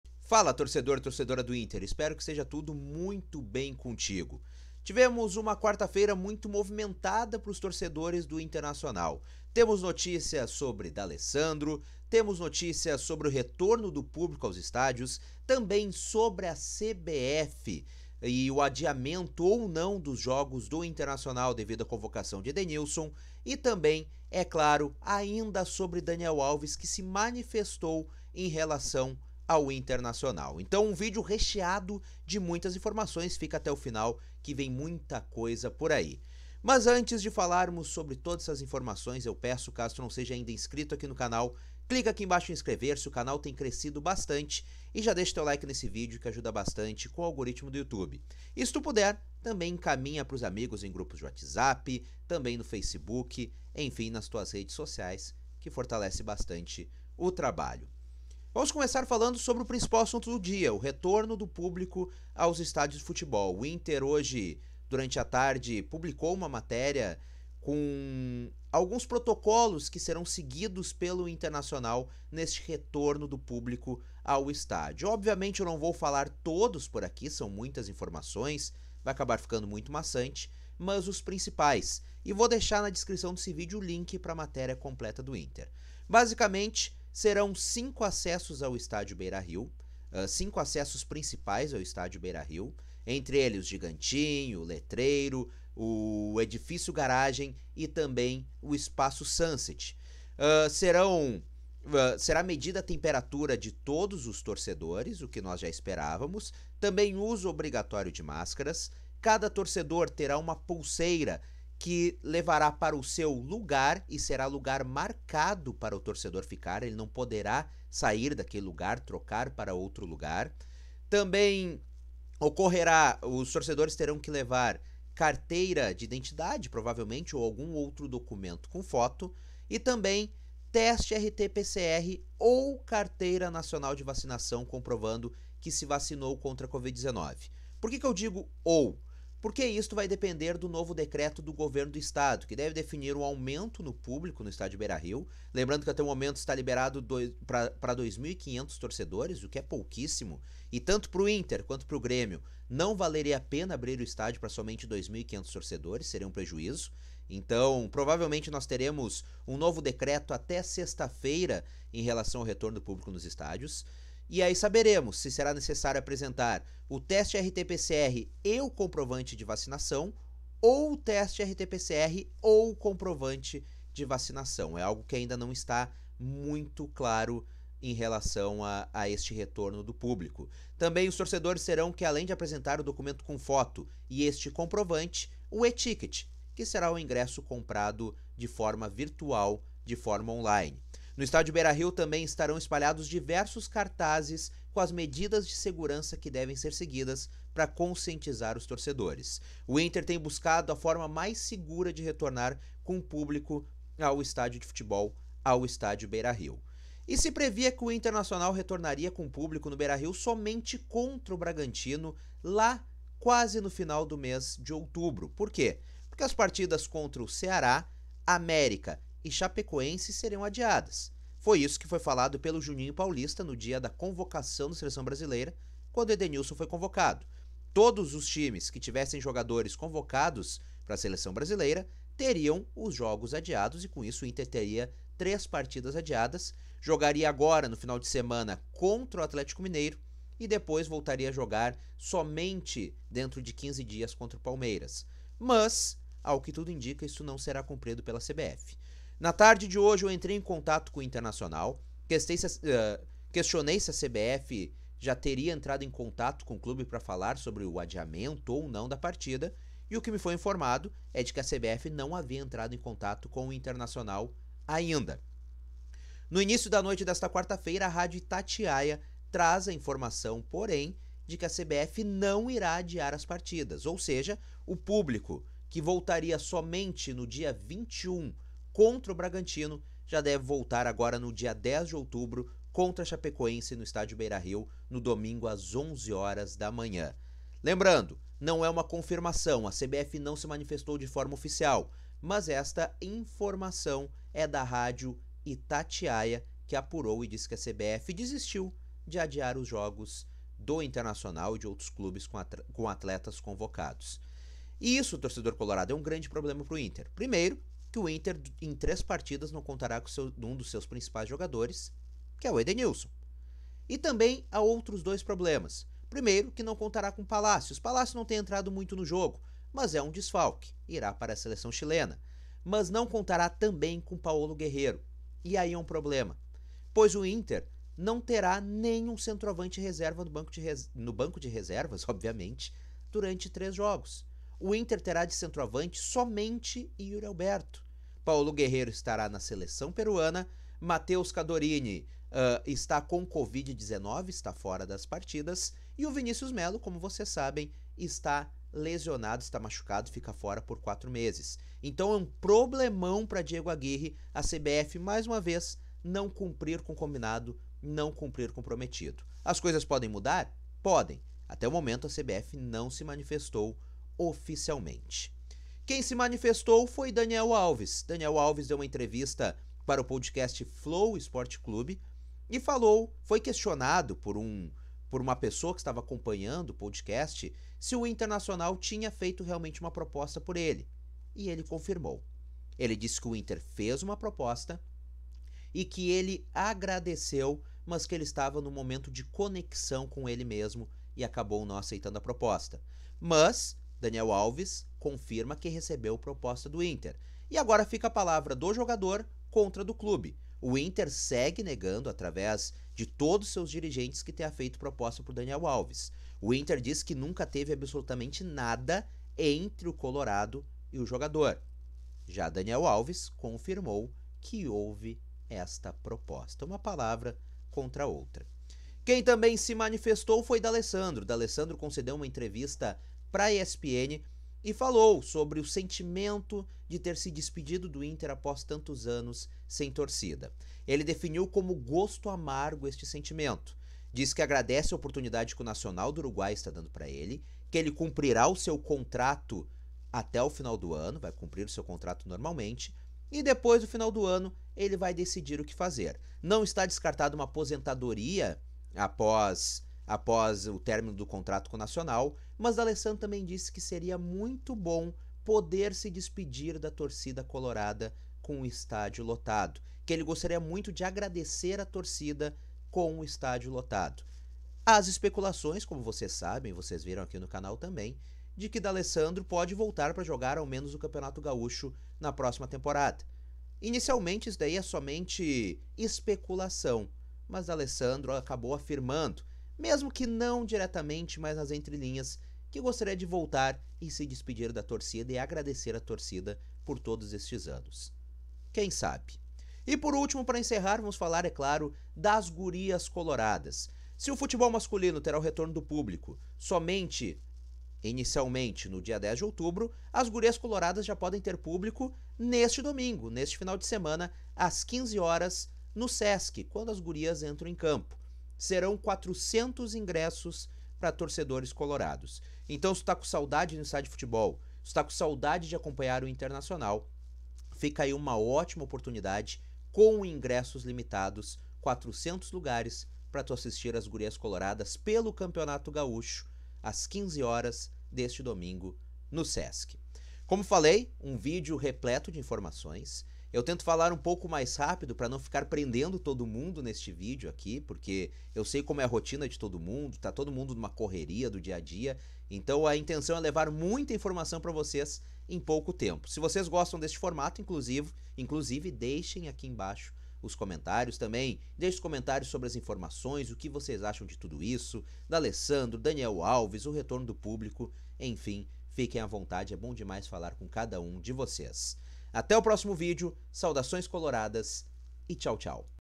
D'ALE QUEBRA SILÊNCIO | DANIEL ALVES FALA SOBRE INTER | RETORNO DO PÚBLICO E CBF
D'Alessandro quebrou o silêncio e falou sobre o futuro de sua carreira.

Daniel Alves confirma proposta do Inter.

Colorado prepara protocolos para retorno do público ao Beira-Rio.